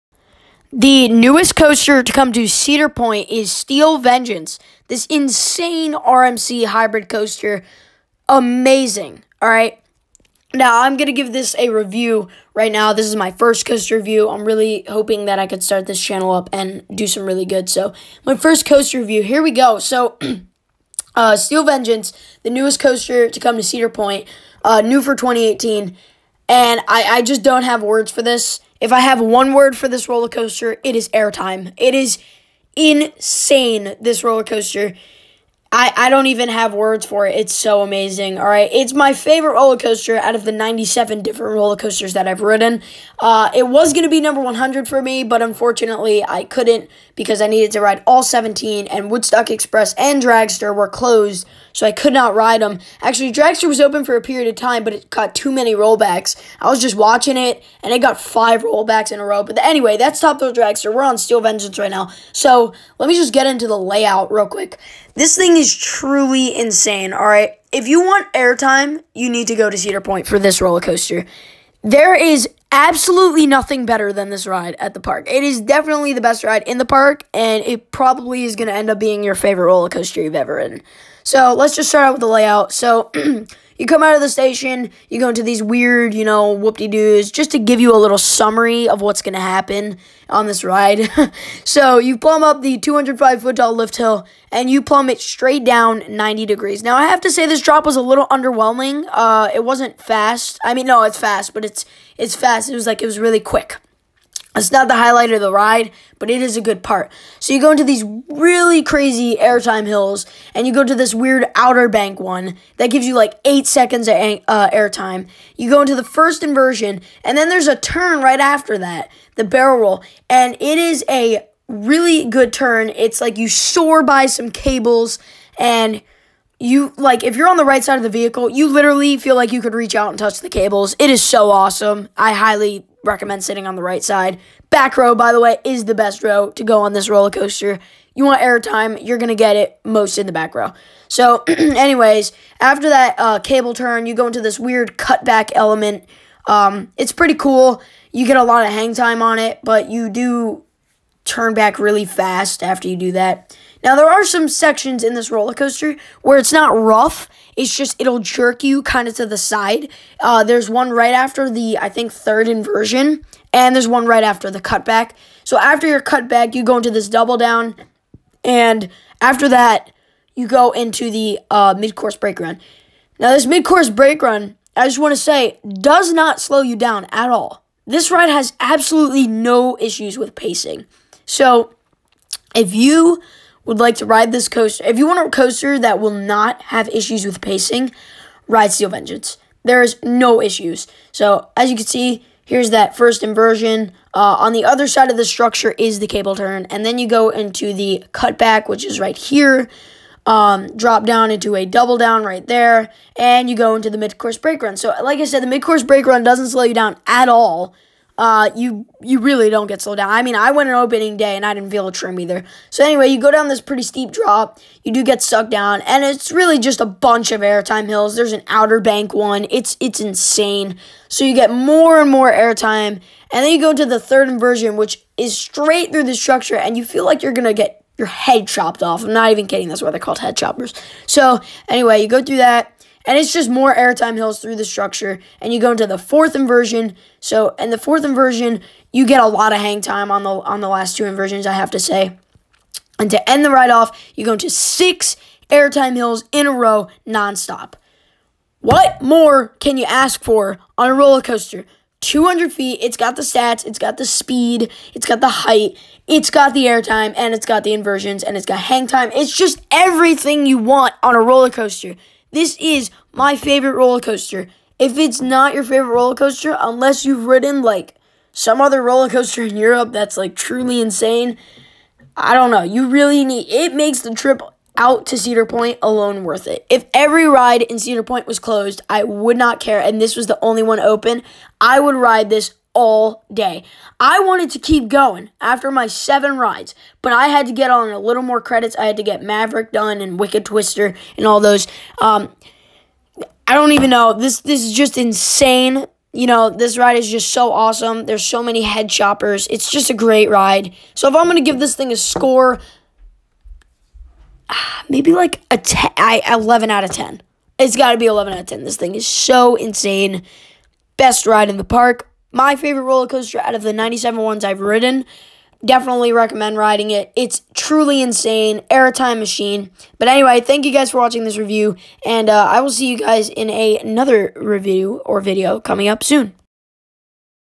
Steel Vengeance Review RMC Hybrid Coaster | Cedar Point
What more can you ask for on a rollercoaster? It’s got the stats, intensity, smoothness, and airtime! This is my favorite rollercoaster I’ve ever ridden, and will probably be yours too! Full in-depth review of Steel Vengeance.

Footage by Coaster Source